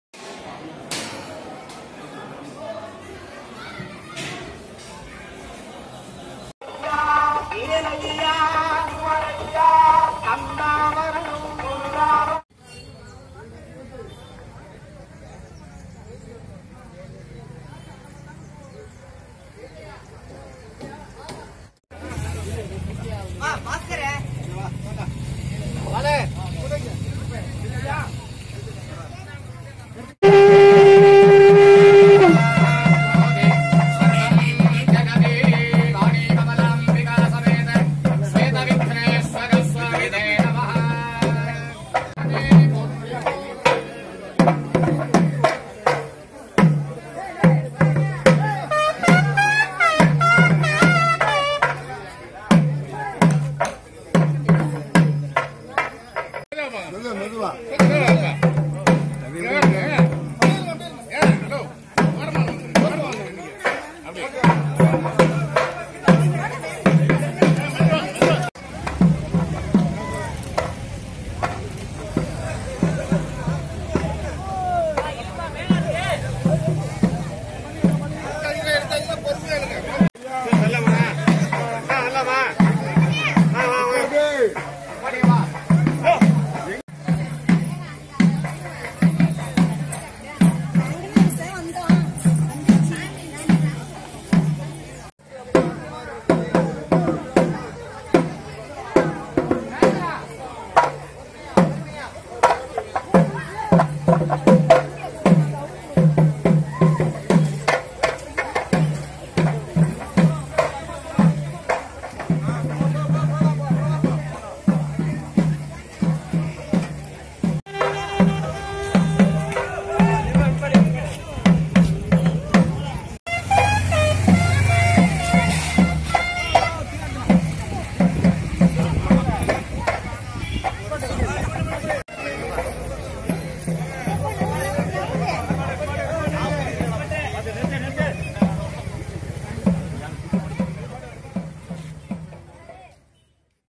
Kumbakonam News Sep 2019 Vinayagar Chathurthi Car Festival @ Thiruvalamsulzhi Temple
Kumbakonam News Sep 2019  Vinayagar Chathurthi Car Festival @ Thiruvalamsulzhi Temple by  Media R Prabakaran on 02.09.19